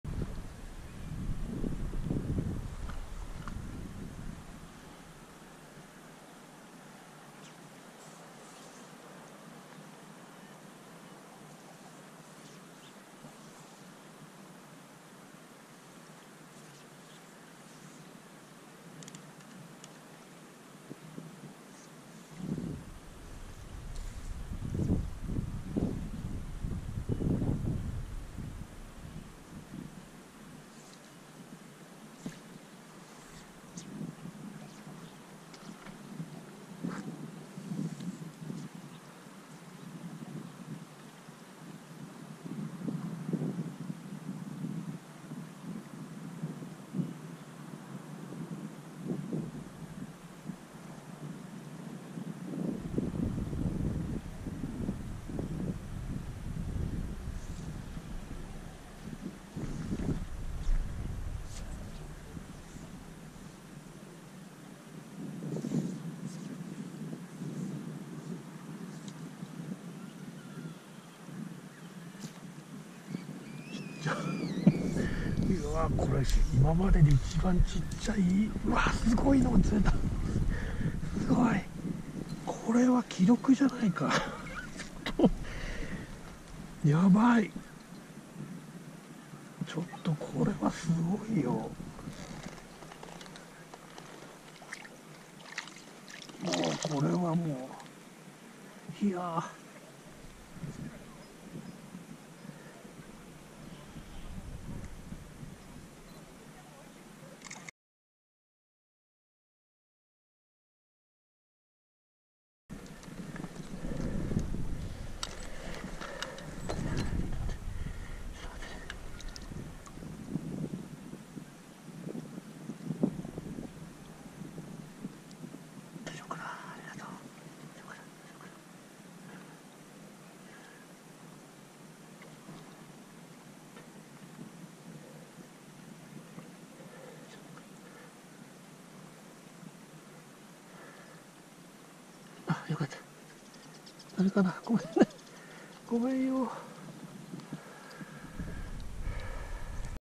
ULTRA MICRO FLY FISHING / 極小フライフィッシング "FFE No.95"
It is the smallest fish I have ever fished.
This is the record fish in my fly fishing experiences.
But this tiny fish is almost dead...I'm sorry from my heart...

3月中旬、近所の川でオイカワ釣り。
いままでで一番小さなオイカワが釣れましたが瀕死の状態・・・
なんとも言えない動画ですが、このオイカワを記録に残したくて、そして
フライフィッシングで極小の魚を釣ることも面白いことを記録したくて
敢えてアップしました。ご不快の場合はどうかご容赦ください。